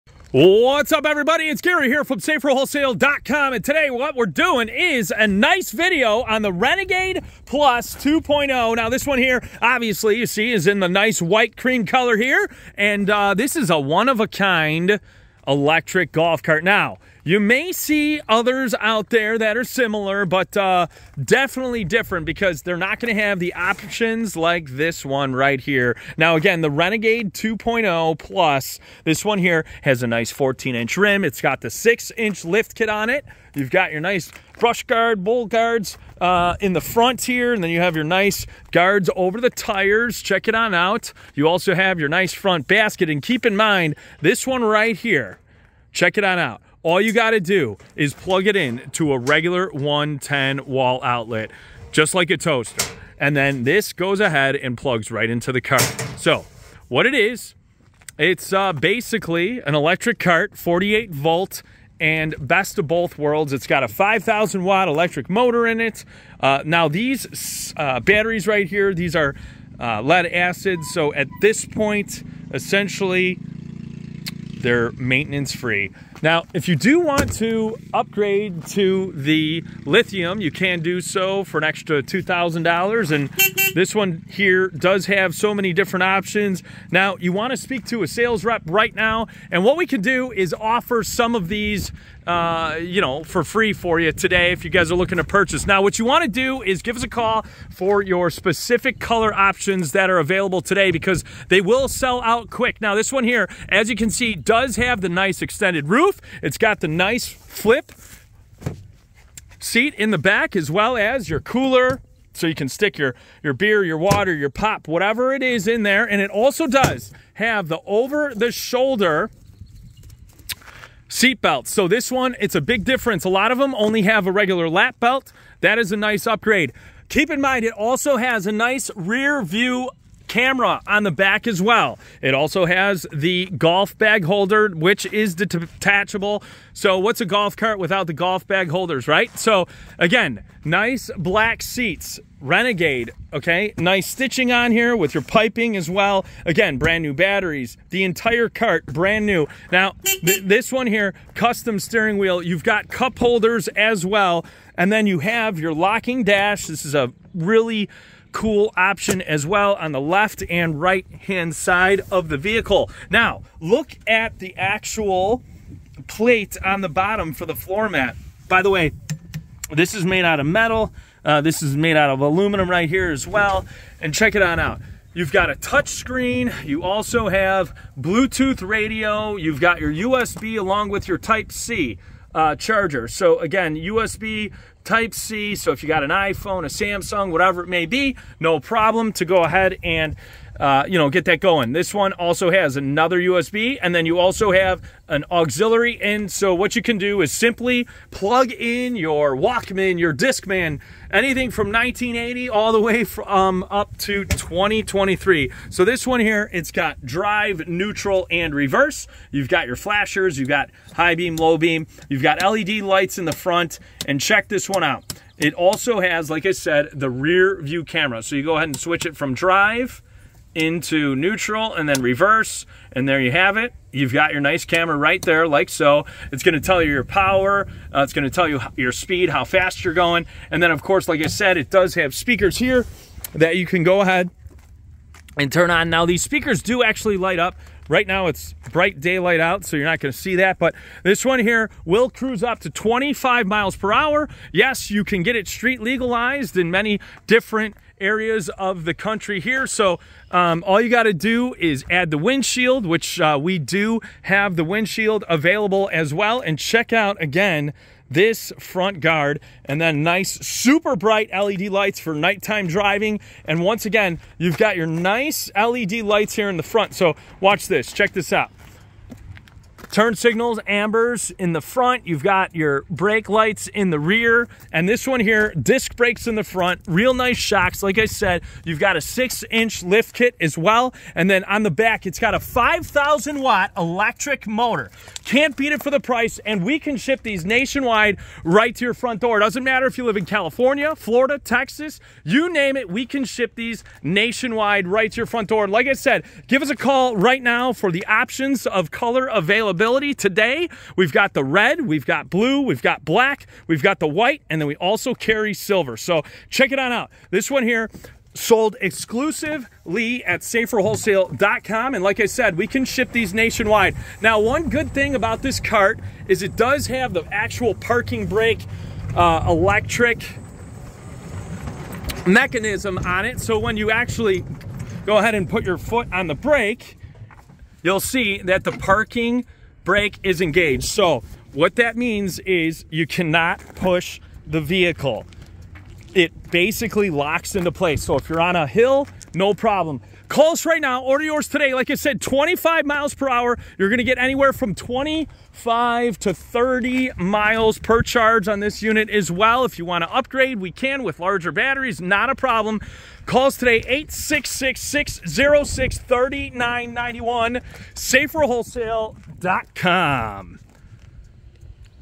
White Creme Renegade Plus 2.0 Electric 48v Golf Cart From SaferWholesale
Check Out The Fully Customizable Renegade Plus 2.0 Electric Golf Cart - Many Available Options Available! Street Legal Capable!